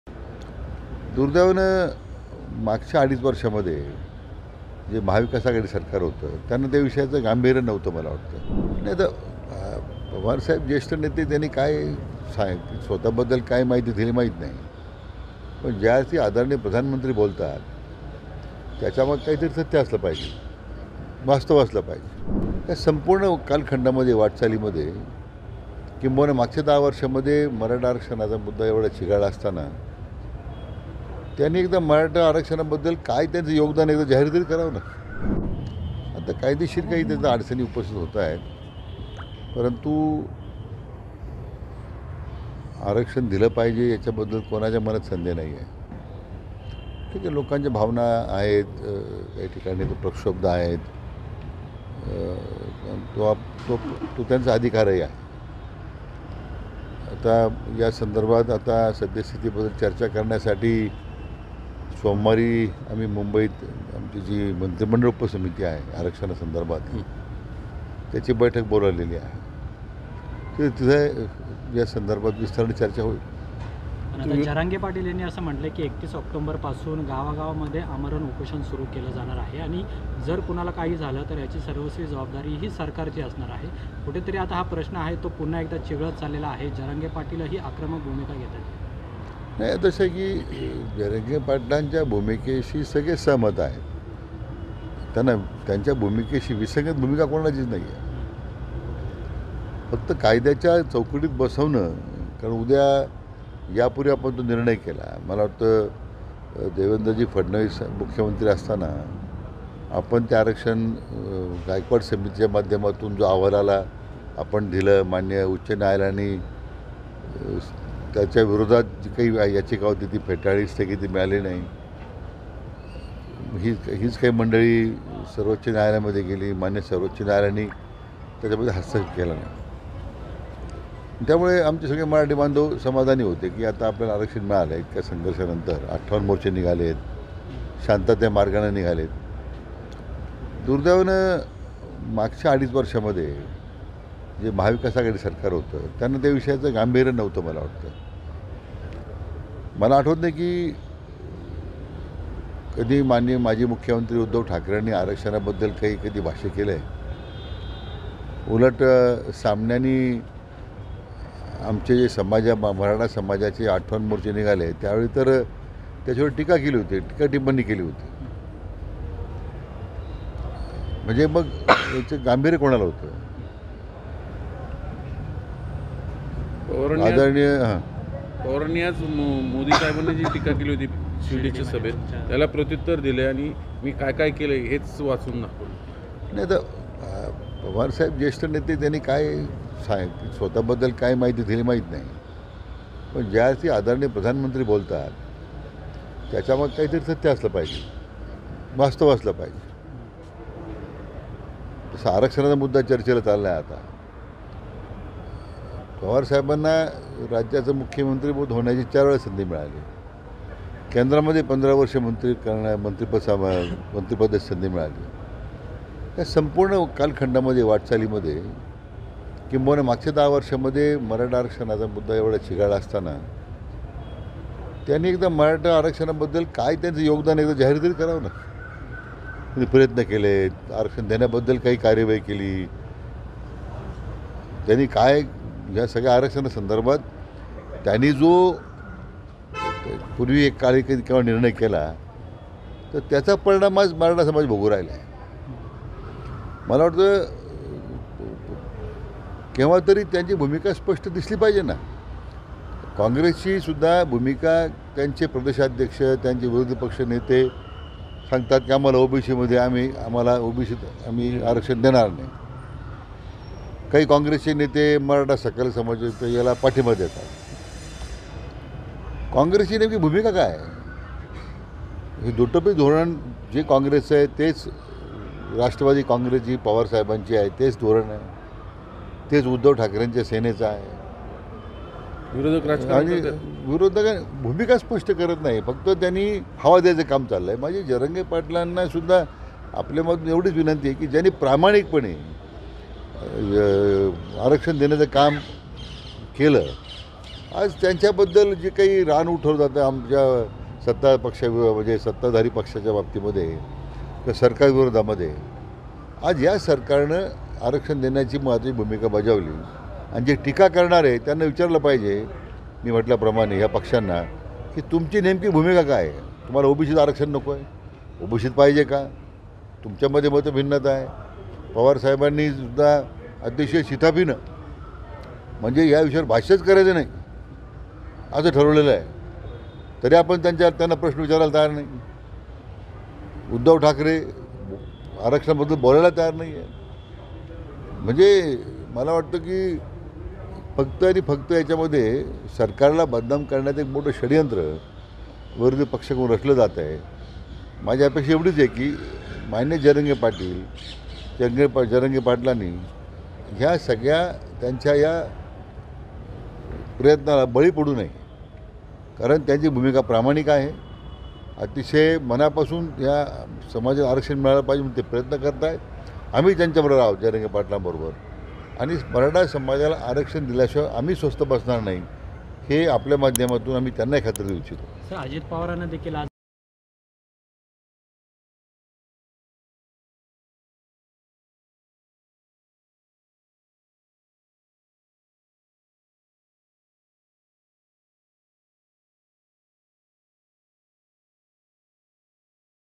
Radhakrishna Vikhe on Pawar : पवारांनी त्यांचं मराठा आरक्षणासाठीचं योगदान जाहीर करावं... | LetsUpp
महसूलमंत्री राधाकृष्ण विखे यांनी मराठा आरक्षणाच्या मुद्द्यावरुन राष्ट्रवादीचे अध्यक्ष शरद पवार यांच्यावर निशाणा साधला.